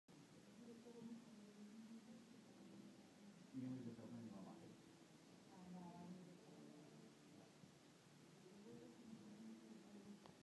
Hampton Bay Minuet II ceiling fan in confession room
UPDATE: this fan was removed as a elevator was installed in it's location, and it was moved into the new confession room that was built.